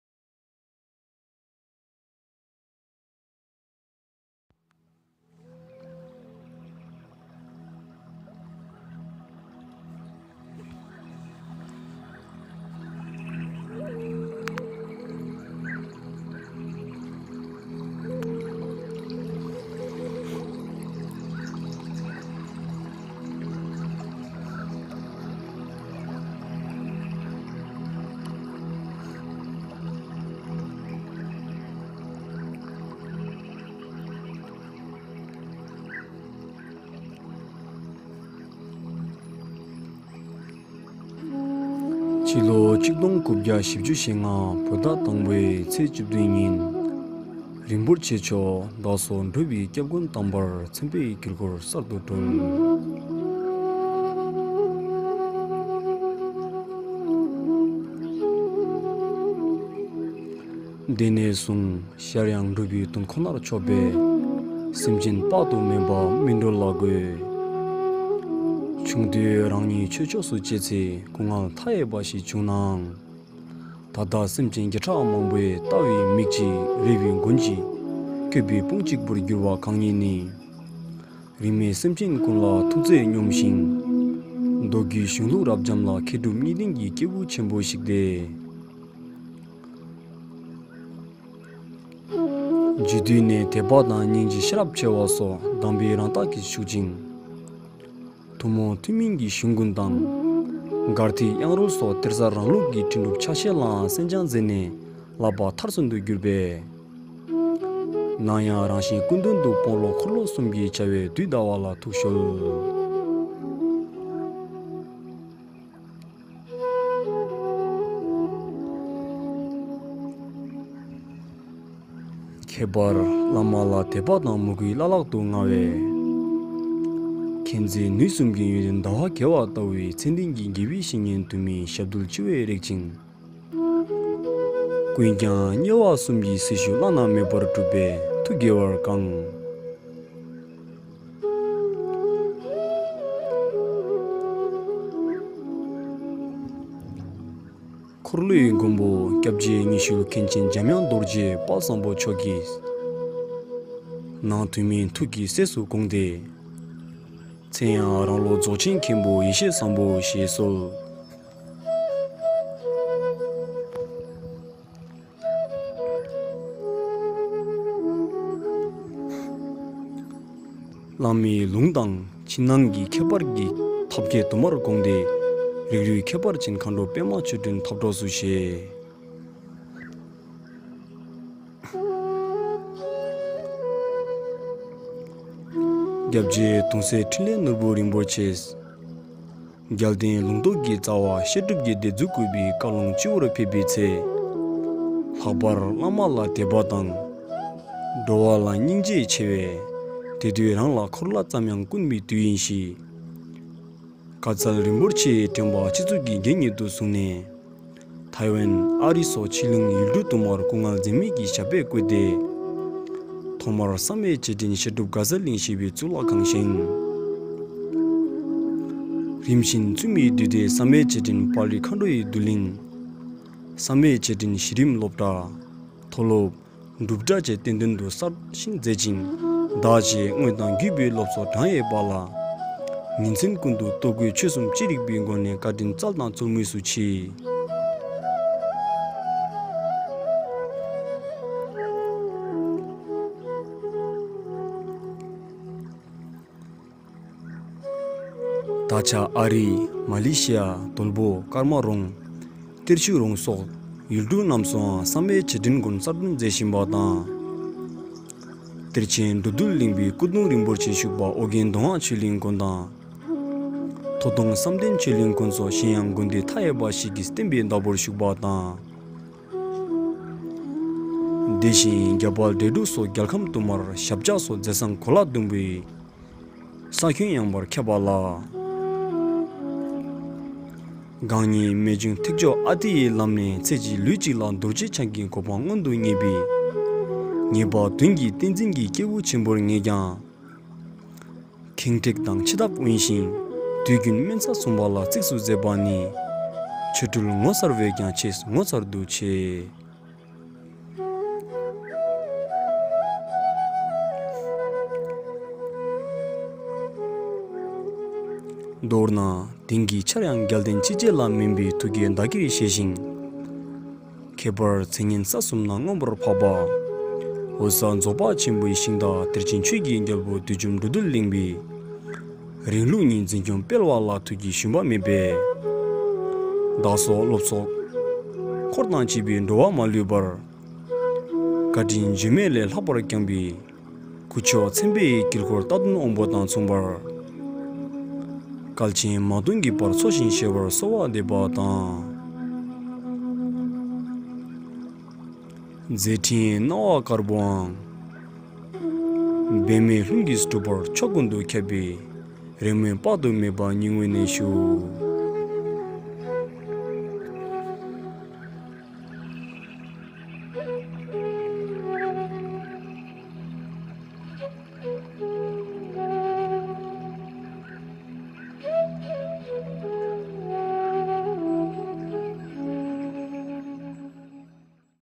An emotional birthday wish video for my root lama Khen Yeshe Sangpo Rinpoche | my first project work
Naration: Tulku Thubten Gelek
Script| Concept| editing: Orgyen Ngodup
Special thanks to  Lopon Jigme Losal & Choe Pema Tsering for helping us to collect photos & videos!

(script)
༡༽ མེ་ཏོག་རྒྱས་བྱེད་ནི་ཉི་མ་རེད། ང་ཚོའི་ཉི་མ་ནི་༧དཔལ་ལྡན་རྩ་བའི་བླ་མ་བཀའ་དྲིན་མཚུངས་ཟླ་མ་མཆིས་པ་ཁྱེད་ཉིད་རེད་ལགས།
༢༽ སྤྱི་ལོ་༡༩༤༥ བོད་ཟླ་༠༡ ཚེས་༡༧ ཉིན་རིན་པོ་ཆེ་མཆོག་བདག་སོགས་འགྲོ་བའི་སྐྱབས་མགོན་དམ་པར་མཚན་དཔེའི་དཀྱིལ་འཁོར་གསར་དུ་གྲོལ།
༣༽ དེ་ནས་བཟུང་ཞར་ཡང་འགྲོ་བའི་དོན་ཁོ་ནར་ཕྱོགས་པས་སེམས་ཅན་དཔག་ཏུ་མེད་པ་སྨིན་གྲོལ་ལ་འགོད། ཆུང་དུས་རང་ཉིད་ཆོས་ཕྱོགས་སུ་སྤྱད་ཚེ་སྐུ་ངལ་མཐའ་ཡས་པ་ཞིག་བྱུང་ནའང་། ད་ལྟ་སེམས་ཅན་བརྒྱ་ཕྲག་མང་པོའི་བལྟ་བའི་མིག་གཅིག རེ་བའི་མགོན་གཅིག སྐྱོབ་པའི་དཔུང་གཅིག་པུར་གྱུར་པ་༧གང་ཉིད་ནི་རིས་མེད་སེམས་ཅན་ཀུན་ལ་ཐུགས་བརྩེ་སྙོམས་ཤིང་། མདོ་རྒྱུད་གཞུང་ལུགས་རབ་འབྱམས་ལ་མཁས་གྲུབ་གཉིས་ལྡན་གྱི་སྐྱེས་བུ་ཆེན་པོ་ཞིག་སྟེ། བྱིས་དུས་ནས་དད་པ་དང་སྙིང་རྗེ་ཤེས་རབ་ཆེ་བ་སོགས་དམ་པའི་རང་རྟགས་ཀྱིས་ཕྱུག་ཅིང་། ཐུན་མོང་ཐུན་མིན་གྱི་གཞུང་ཀུན་དང་། གར་ཐིག་དབྱངས་རོལ་སོགས་གཏེར་གསར་རང་ལུགས་ཀྱི་འཕྲིན་སྒྲུབ་ཕྱག་བཞེས་ལའང་གསན་སྦྱངས་མཛད་ནས་བསླབ་པ་མཐར་སོན་དུ་གྱུར་པས། ནམ་ཡང་རང་གཞན་ཀུན་དོན་དུ་སྤོང་ཀློག་འཁོར་ལོ་གསུམ་གྱི་བྱ་བས་དུས་འདའ་བ་ལ་ཐུགས་གཞོལ།
༤༽ ཁྱད་པར་བླ་མ་ལ་དད་པ་དང་མོས་གུས་བླ་ལྷག་ཏུ་མངའ་བས། མཁྱེན་བརྩེ་ནུས་གསུམ་གྱི་ཡོན་ཏན་ཟླ་བ་རྒྱས་པ་ལྟ་བུའི་ཚད་ལྡན་གྱི་དགེ་བའི་བཤེས་གཉེན་དུ་མའི་ཞབས་རྡུལ་སྤྱི་བོས་རེག་ཅིང་། ཀུན་ཀྱང་མཉེས་པ་གསུམ་གྱི་སྲི་ཞུ་བླ་ན་མེད་པར་བསྒྲུབས་པས་ཐུགས་དགྱེས་པར་བསྐངས། ༧འཁོར་ལོའི་མགོན་པོ་༧སྐྱབས་རྗེ་སྨྱོ་ཤུལ་མཁན་ཆེན་འཇམ་དབྱངས་རྡོ་རྗེ་དཔལ་བཟང་པོ་མཆོག་གིས་ནང་ཐུན་མིན་ཐུགས་ཀྱི་སྲས་སུ་དགོངས་ཏེ། མཚན་ཡང་རང་སློབ་༧རྫོགས་ཆེན་མཁན་པོ་ཡེ་ཤེས་བཟང་པོ་ཞེས་གསོལ།
 བླ་མའི་ལུང་དང་ཕྱི་ནང་གི་ཁྱད་པར་གྱི་ཐབས་ཁྱད་དུ་མར་དགོངས་ཏེ་རིགས་རུས་ཁྱད་པར་ཅན་༧མཁའ་འགྲོ་པདྨཆོས་སྒྲོན་ཐབས་གྲོགས་སུ་བཞེས།
༥༽ ༧སྐྱབས་རྗེ་གདུང་སྲས་ཕྲིན་ལས་ནོར་བུ་རིན་པོ་ཆེས། རྒྱལ་བསྟན་ལུང་རྟོགས་ཀྱི་རྩ་བ་བཤད་སྒྲུབ་ཀྱི་སྡེ་འཛུགས་དགོས་པའི་བཀའ་ལུང་སྤྱི་བོར་ཕེབས་པའི་ཚེ། ལྷག་པར་བླ་མ་ལ་དད་པ་དང་འགྲོ་བ་ལ་སྙིང་རྗེ་ཆེ་བས་དེ་དུས་རང་ལ་འཁོར་གླ་ཙམ་ཡང་དཀོན་པའི་དུས་ཡིན་གཤིས་བཀའ་སྩལ་རིན་པོ་ཆེའི་ཕྲེང་བ་སྤྱི་གཙུག་གི་རྒྱན་ཉིད་དུ་བཟུང་ནས། ཐཻ་ཝན། ཨ་རི་སོགས་ཕྱི་གླིང་ཡུལ་གྲུ་དུ་མར་སྐུ་ངལ་འཛེམས་མེད་ཀྱིས་ཞབས་པད་བཀོད་དེ་ཐོག་མར་བསམ་ཡས་རྗེས་དྲན་བཤད་སྒྲུབ་དགའ་ཚལ་གླིང་ཞེས་པའི་གཙུག་ལག་ཁང་བཞེངས།  རིམ་བཞིན་བཙུན་མའི་འདུས་སྡེ་བསམ་ཡས་རྗེས་དྲན་དཔལ་རི་མཁའ་འགྲོའི་འདུ་གླིང་། བསམ་ཡས་རྗེས་དྲན་གཞི་རིམ་སློབ་གྲྭ། མཐོ་སློབ། སྒྲུབ་གྲྭ་བཅས་བསྟན་དོན་དུ་གསར་བཞེངས་མཛད་ཅིང་བདག་བཅས་དངོས་དང་བརྒྱུད་པའི་སློབ་ཚོགས་བགྲང་ཡས་པ་ལ་ཉིན་མཚན་ཀུན་ཏུ་ལྟོ་གོས་ཆོས་གསུམ་ཅི་རིགས་པའི་སྒོ་ནས་བཀའ་དྲིན་བསྩལ་དང་སྩོལ་མུས་སུ་མཆིས།
༦༽ ད་ཆ་ཨ་རི། མ་ལེ་ཤི་ཡ། དོལ་པོ། དཀར་མ་རོང་། གཏེར་ཆུ་རོང་སོགས་ཡུལ་གྲུ་རྣམས་སུའང་བསམ་ཡས་རྗེས་དྲན་དགོན་གསར་བསྐྲུན་མཛད་བཞིན་པ་དང་། ༧གཏེར་ཆེན་འགྲོ་འདུལ་གླིང་པའི་སྐུ་གདུང་རིན་པོ་ཆེ་བཞུགས་པ་ཨོ་རྒྱན་མདོ་སྔགས་ཆོས་གླིང་དགོན་དང་། མཐོ་མཐོང་བསམ་གཏན་ཆོས་གླིང་དགོན་སོགས་གཞན་ཡང་དགོན་སྡེ་མཐའ་ཡས་པ་ཞིག་གི་བསྟན་པའི་བདག་པོར་བཞུགས་པ་དང་། དེ་བཞིན་རྒྱ་བལ་འབྲས་འབྲུག་སོགས་རྒྱལ་ཁམས་དུ་མར་ཞབས་བཅག་སོགས་མཛད་བཟང་གོ་ལ་ཟླུམ་པོའི་ཁྱོན་ཡངས་པར་ཁྱབ་པ་ལགས།
༧༽ ༧གང་ཉིད་རྨད་བྱུང་ཐེག་མཆོག་ཨ་ཏིའི་ལམ་ནས་ཚེ་གཅིག་ལུས་གཅིག་ལ་རྡོ་རྗེ་འཆང་གི་གོ་འཕང་མངོན་དུ་བརྙེས་པའི་ངེས་པ་དོན་གྱི་བསྟན་འཛིན་གྱི་སྐྱེས་བུ་ཆེན་པོར་ངེས་ཀྱང་། ཁེངས་དྲེགས་དང་ཆེ་ཐབས་དབེན་ཞིང་དུས་རྒྱུན་དམན་ས་བཟུང་བ་ལ་གཙིགས་སུ་མཛད་པ་ནི་ཆོ་འཕྲུལ་ངོ་མཚར་བས་ཀྱང་ཆེས་ངོ་མཚར་དུ་ཆེ།
 མདོར་ན་དེང་གི་ཆར་ཡང་རྒྱལ་བསྟན་སྤྱི་བྱེ་ལ་སྨན་པའི་ཐུགས་བསྐྱེད་བདག་གིར་བཞེས་ཤིང་། ཁྱད་པར་མཚན་སྙན་ས་གསུམ་ན་མངོན་པར་འཕགས་པ་འོད་གསལ་རྫོགས་པ་ཆེན་པོའི་ཤིང་རྟ་གཏེར་ཆེན་ཆོས་ཀྱི་རྒྱལ་པོ་བདུད་འཇོམས་འགྲོ་འདུལ་གླིང་པའི་རིང་ལུགས་ཉིད་འཛིན་སྐྱོང་སྤེལ་བ་ལ་ཐུགས་བསྐྱེད་ཞུམ་པ་མེད་པས། བདག་སོགས་སློབ་ཚོགས་བཅས་པའི་འགྲོ་བ་མ་ལུས་པར་བཀའ་དྲིན་བརྗོད་མེད་ལས་ལྷག་པར་བསྐྱངས་པའི་༧སྐུ་མཆོག་མཚན་དཔེའི་དཀྱིལ་འཁོར་རྟ་བདུན་དབང་པོ་དང་མཚུངས་པར་བསྐལ་ཆེན་མ་སྟོང་གི་བར་འཚོ་ཞིང་གཞེས་པར་གསོལ་བ་འདེབས་པ་དང་། མཛད་ཕྲིན་སྣང་བ་དཀར་པོའང་འབད་མེད་ལྷུན་གྱིས་གྲུབ་པར་ཕྱོགས་ཀུན་ཏུ་ཁྱབ་པའི་རེ་སྨོན་དཔག་ཏུ་མེད་པ་སྙིང་དབུས་ནས་ཞུུ།

Samye Memorial Institute